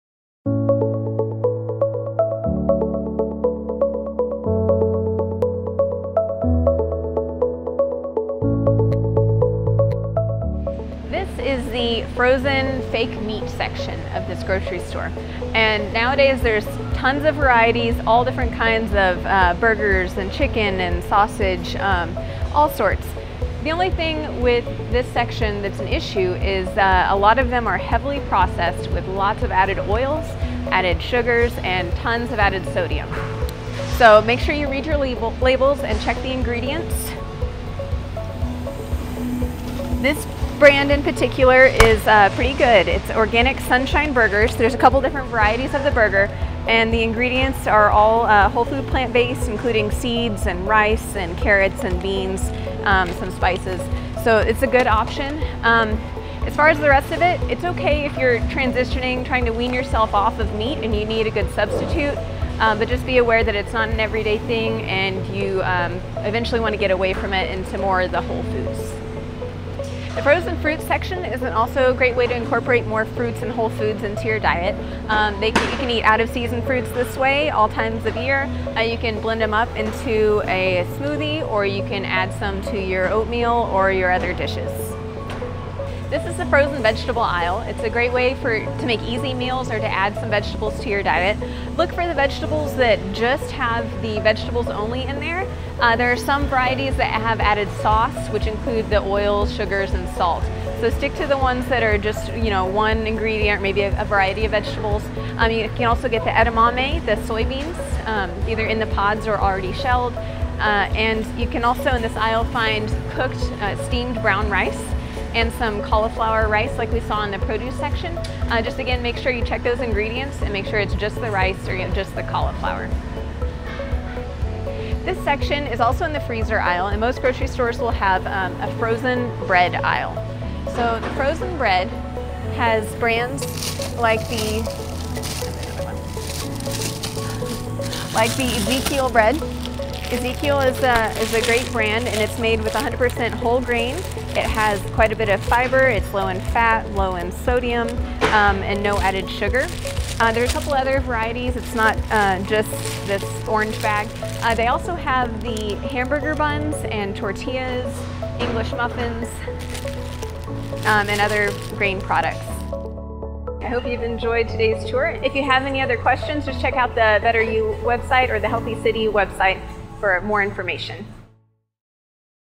Grocery Store Tour Part 2: Frozen Foods
Join us for Part 2 of our Grocery Store Tour - the frozen foods section!